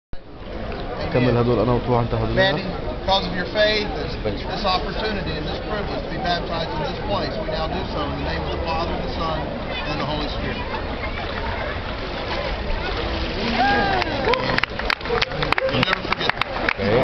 Baptism in the River Jordan, Israel
Baptism in the River Jordan in Israel, just south of the Sea of Galilee, March 20, 2008